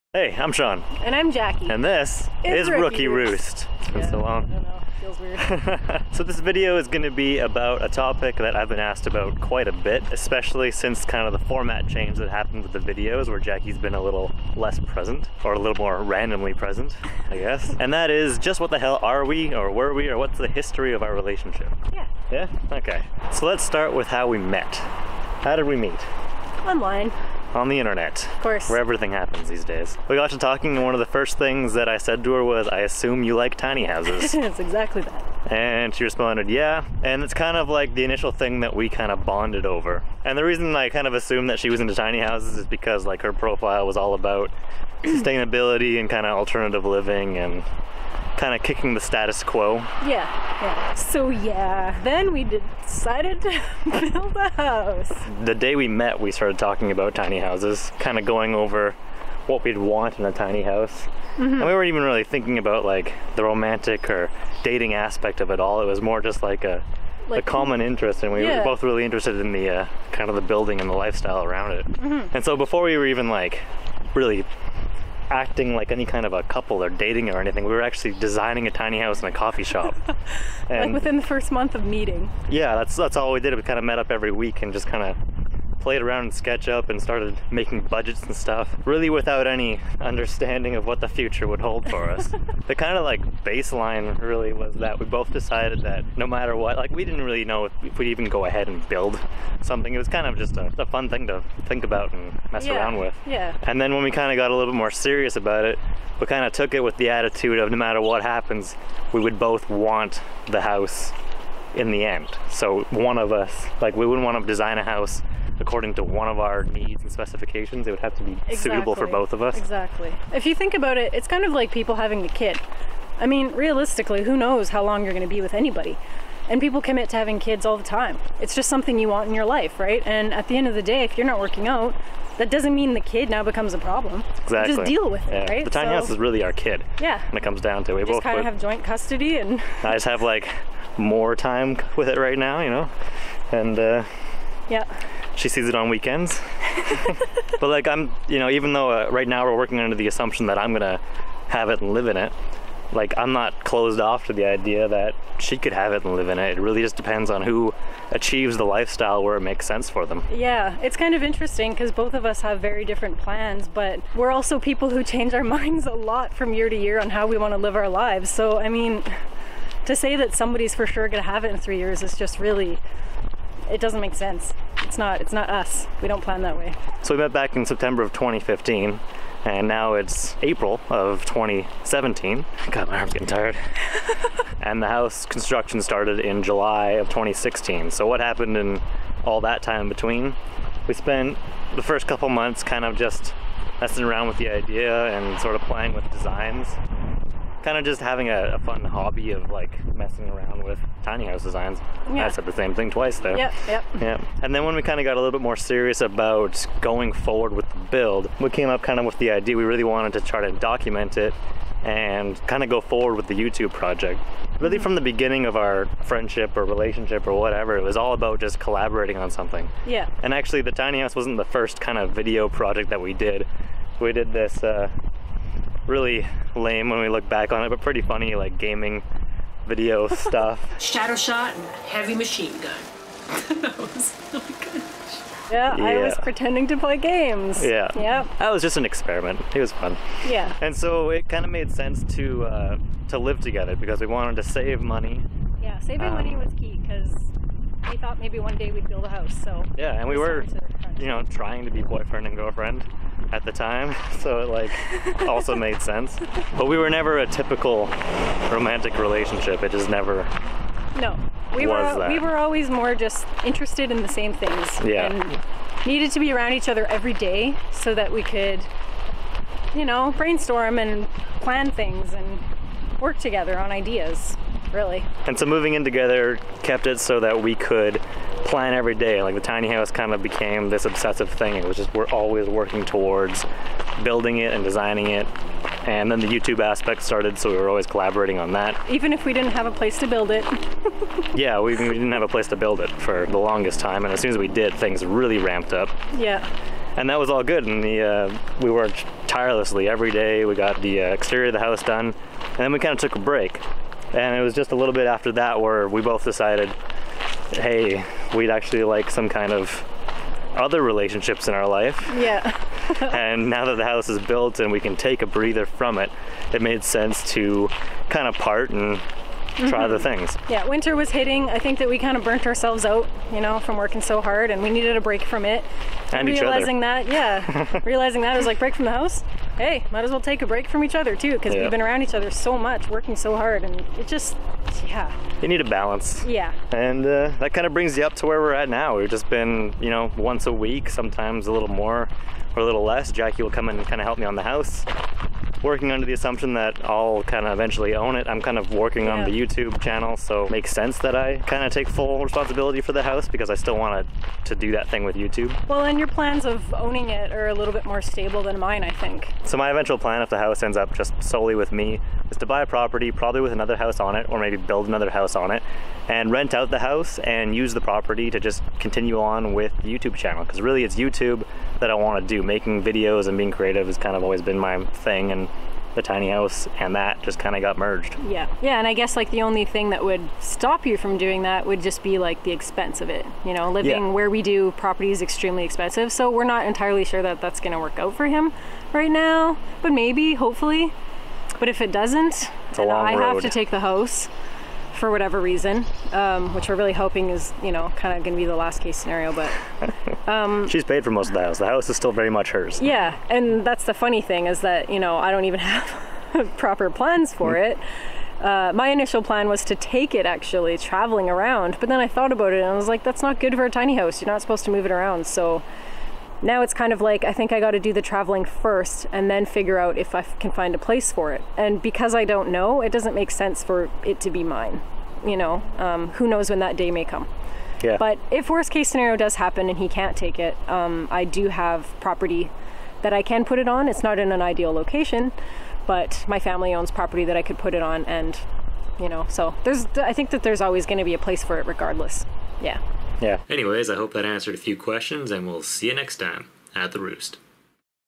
Tiny House Break Up?
A casual video where we talk about the nature and history of our relationship. Many questions have been asked, and we hope this helps to answer some of them.